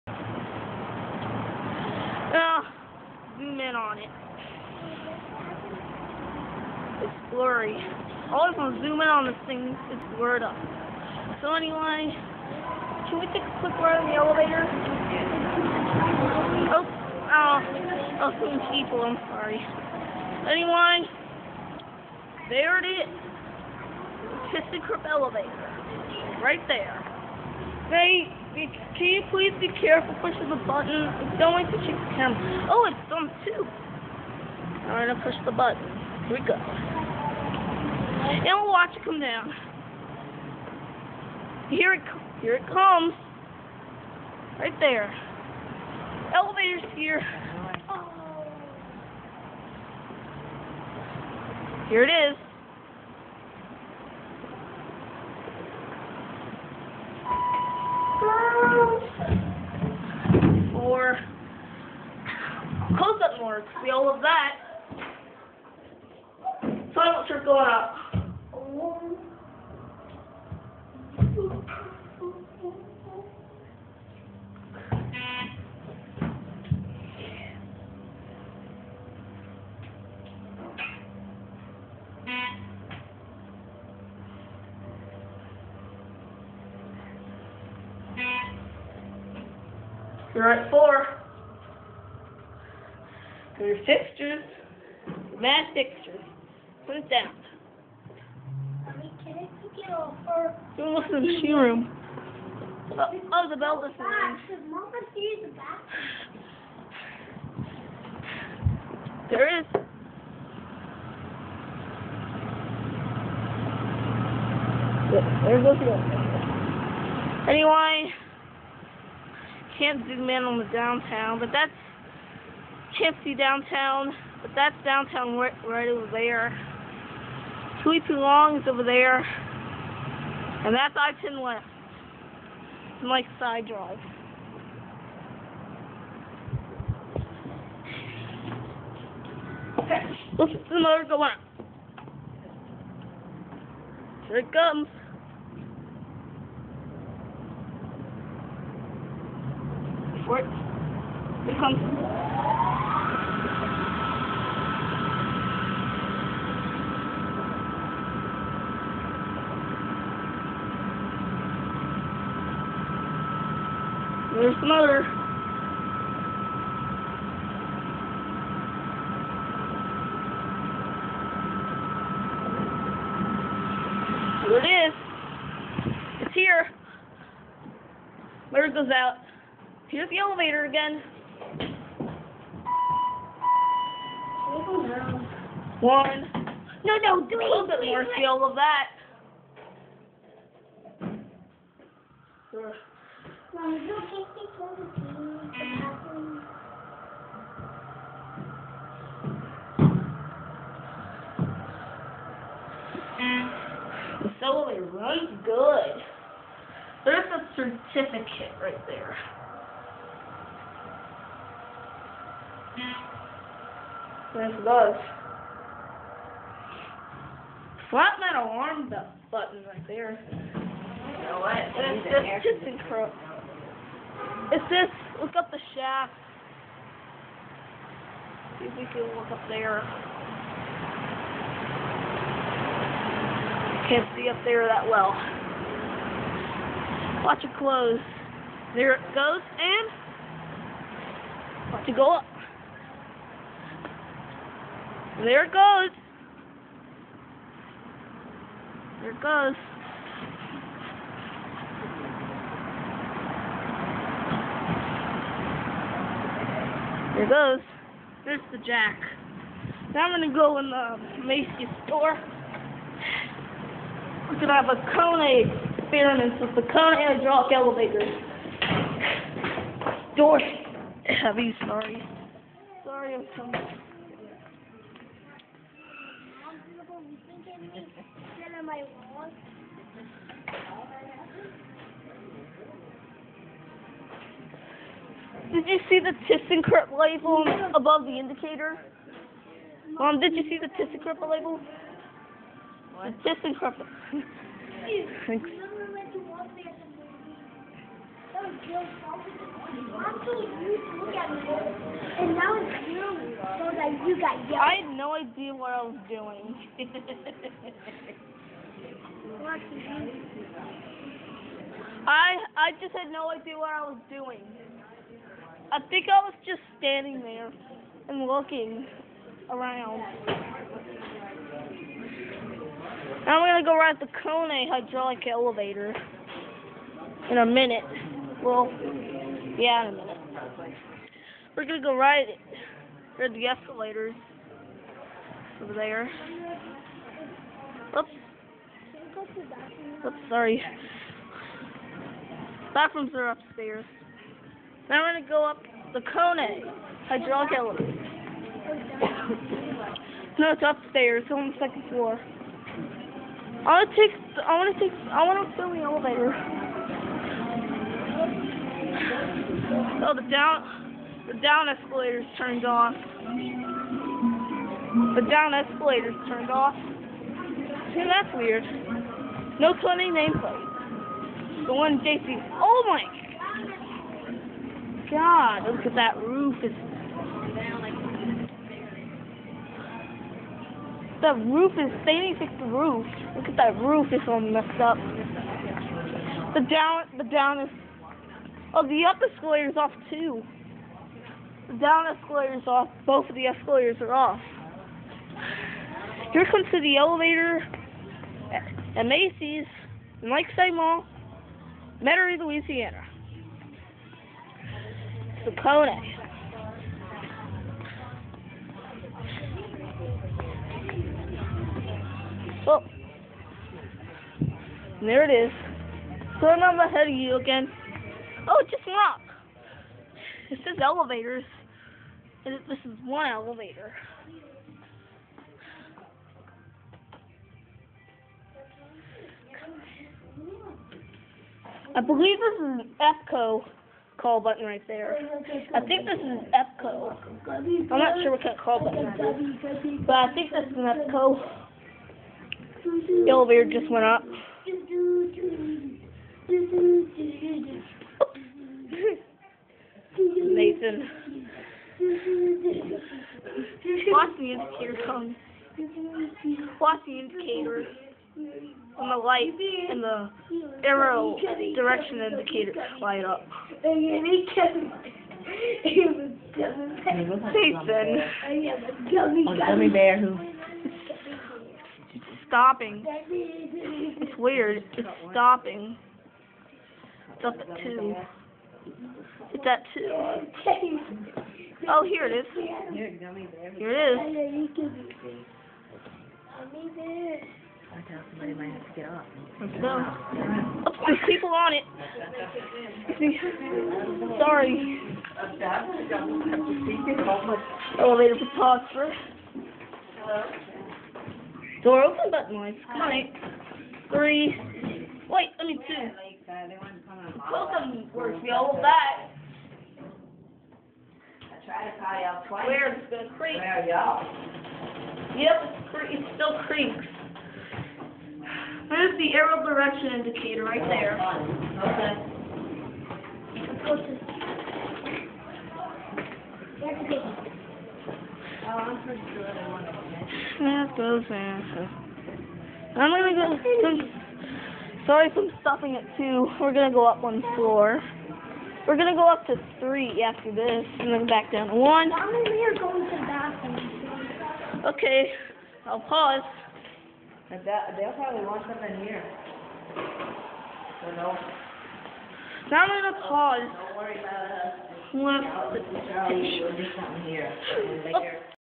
ThyssenKrupp/Kone Hydraulic elevators at Macy's Parking Garage/store ;at Lakeside Mall in Metairie.
This is another Mall in Metairie LA.But tis Mall has more elevators.